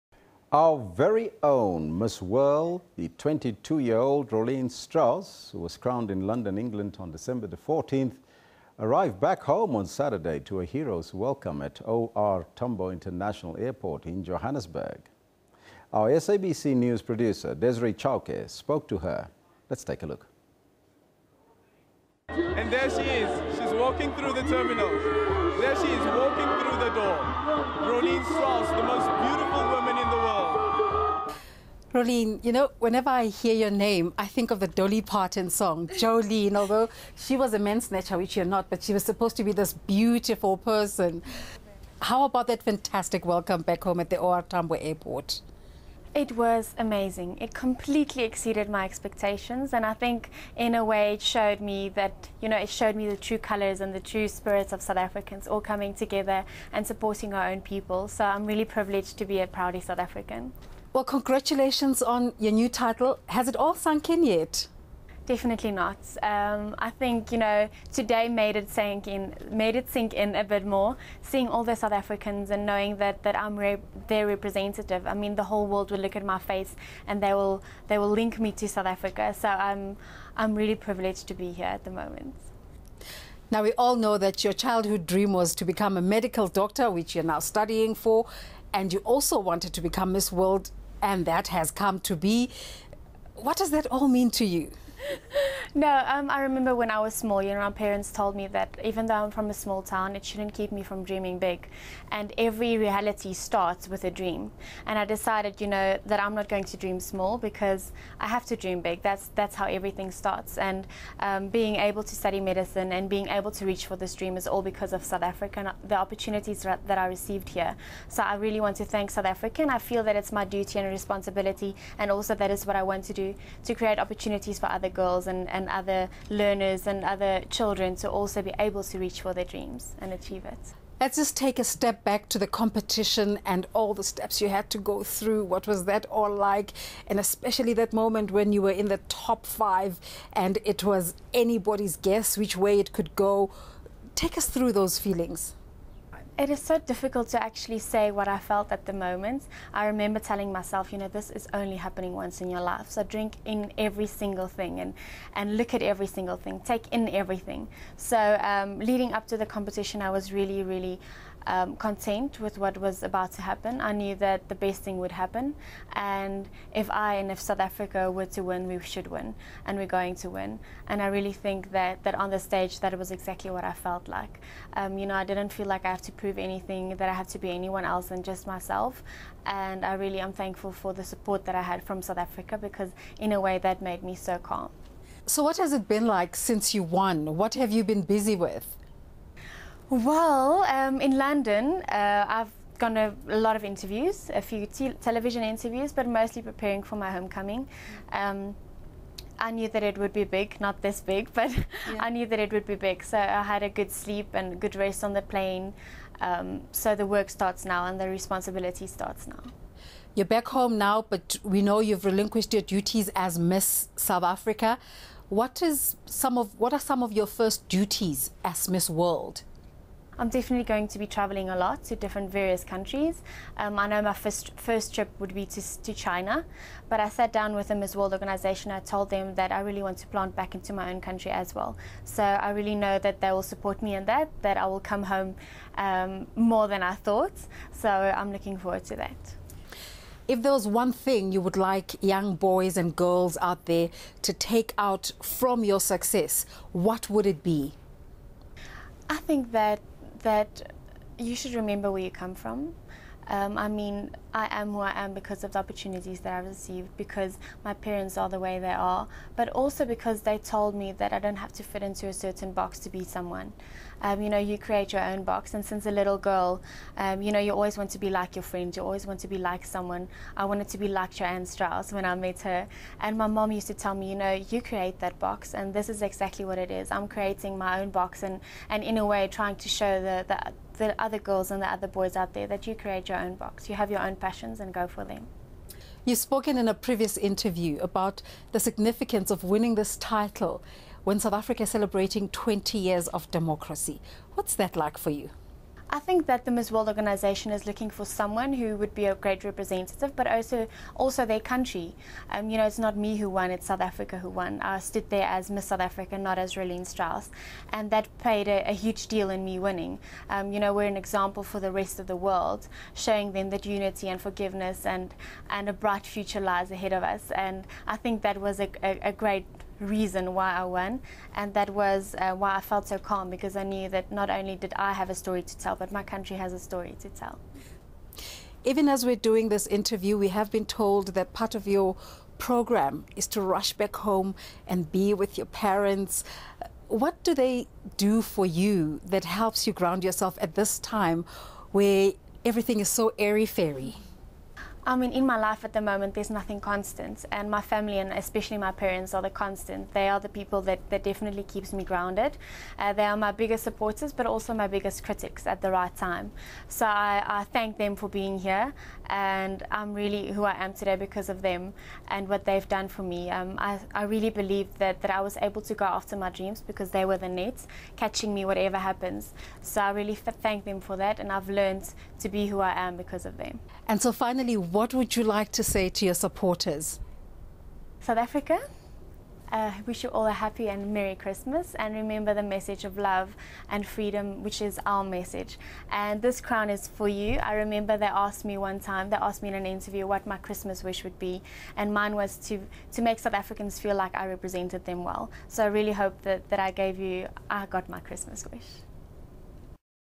Rolene Strauss on being crowned Miss World
Our very own Miss World 22 year old, Rolene Strauss, who was crowned in London, England on December 14 and arrived back home on Saturday to a hero's welcome at OR Tambo International Airport in Johannesburg.